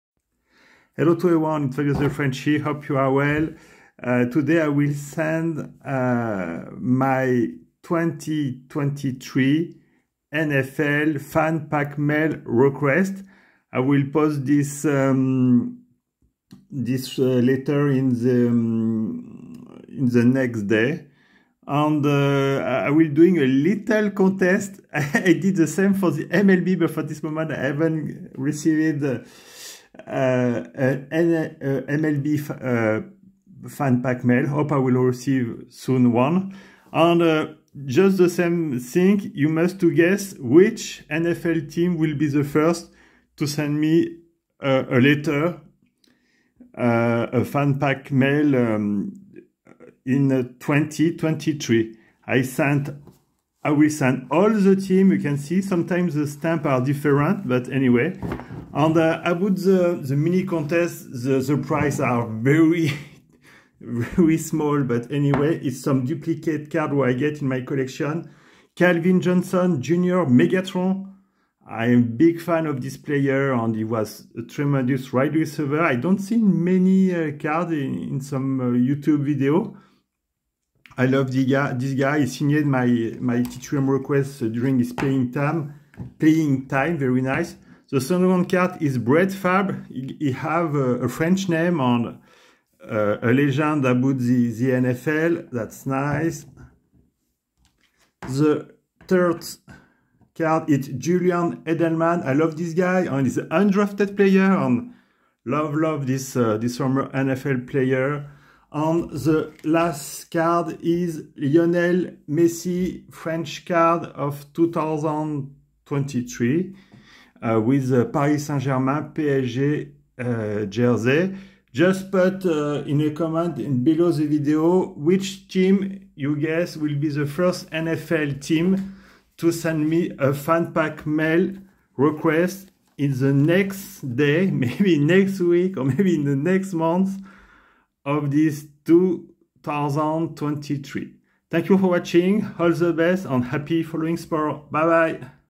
i just sent some fan pack mail requests to all the NFL teams + a mini contest announcement
Rules of the contest :
- put in the comments section, the name of the NFL  franchise whose you guess  will be the first franchise in 2023  to send me a letter/fan pack mail
- only the people who  had posted a comment on my Youtube Channel before i edited this video, can enter in this contest
- if 2 or more guys have guessed the right team, i will doing a draw or maybe i will divide the gift prize.
- you can enter in the contest until i not yet edited my first 2023 NFL fan pack mail return
- the contest wil end when i will receive my first 2023 NFL fan pack mail return